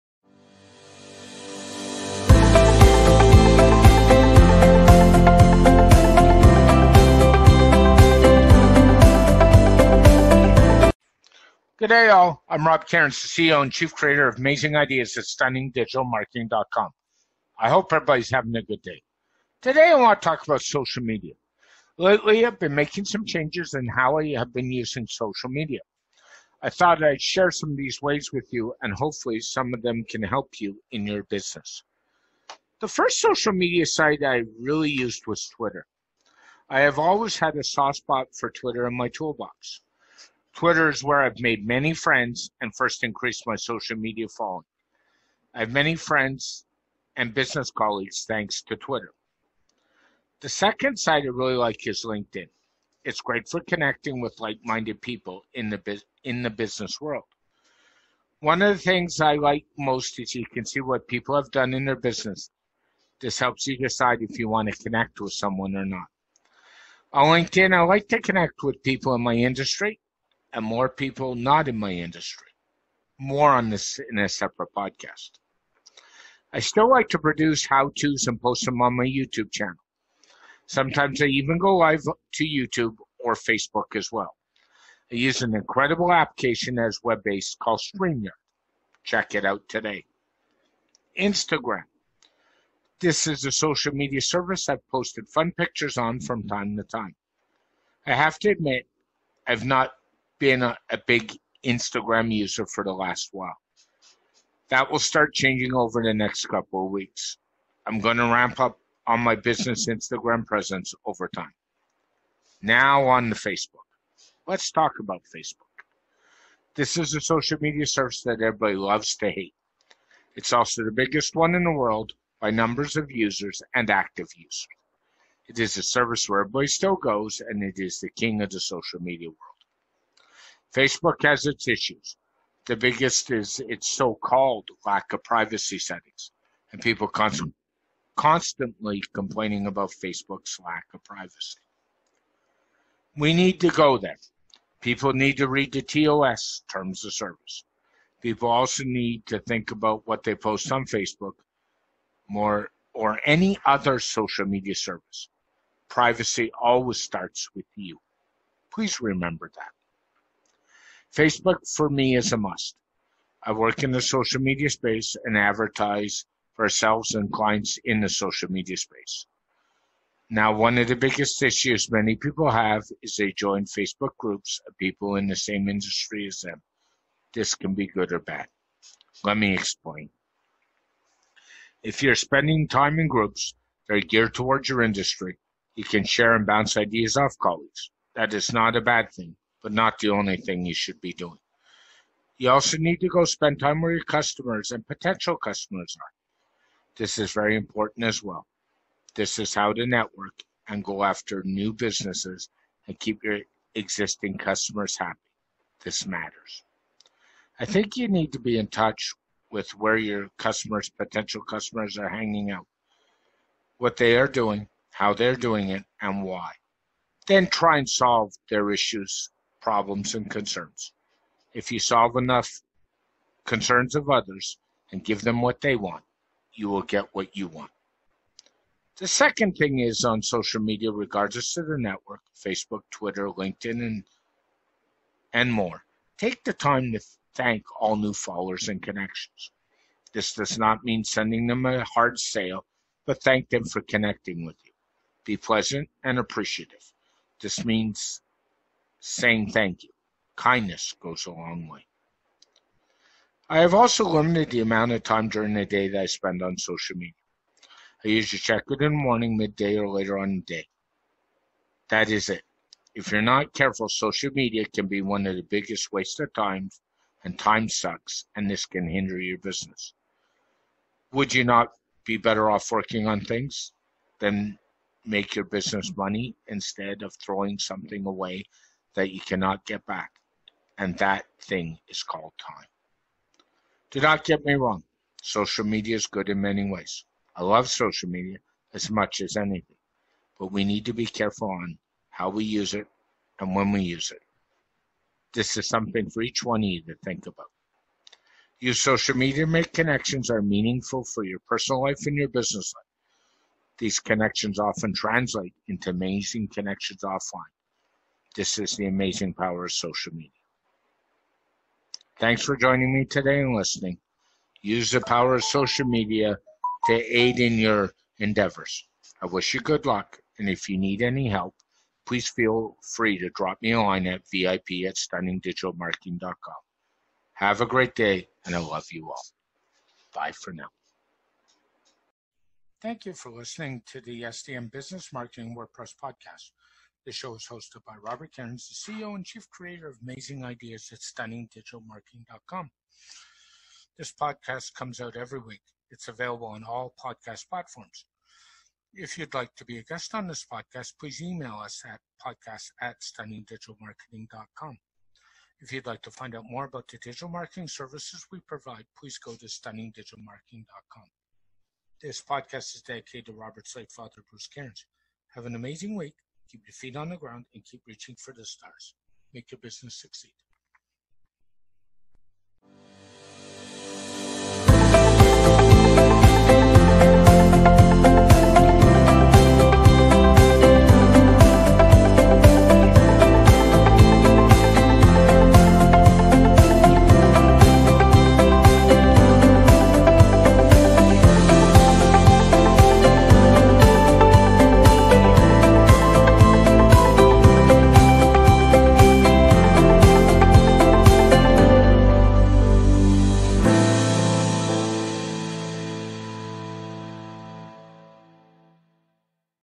The SDM Show: Episode 37 Why I Have Changed How and When I Use Social Media
Robert Cairns the CEO/Chief Creator of Amazing Ideas at StunningDigitalMarketing.com talks about how he has changed his use of social media. These changes come from over 10 years of experience on several social media platforms.